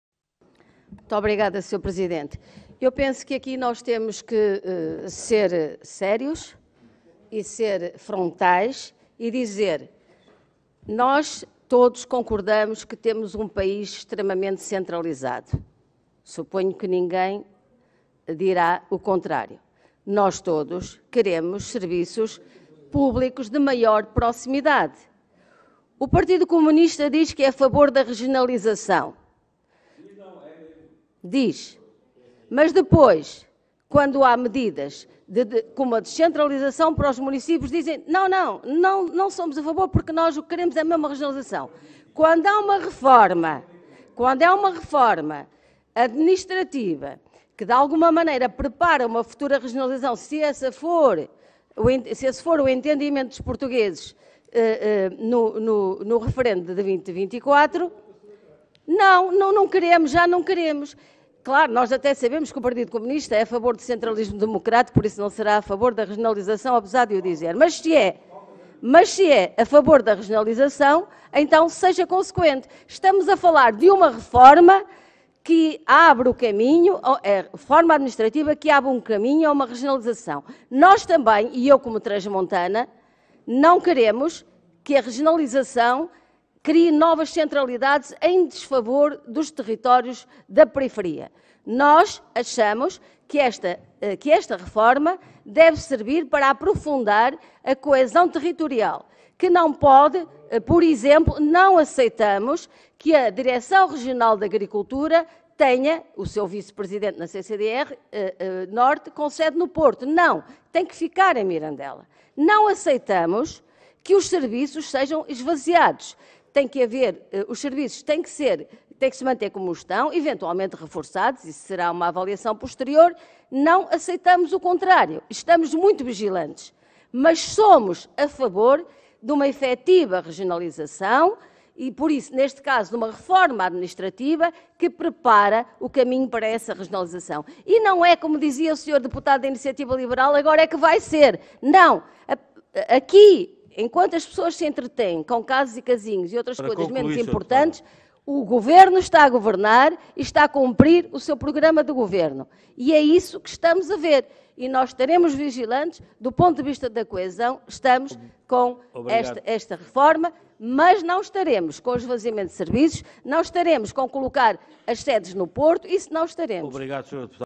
18-01-2023 | Audição da Ministra da Agricultura e Alimentação | Berta Nunes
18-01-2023 | Intervenção da deputada Berta Nunes - Audição da Ministra da Agricultura e Alimentação, sobre as Direções Regionais de Agricultura (requerimentos do CH e da IL) - Comissão de Agricultura e Pescas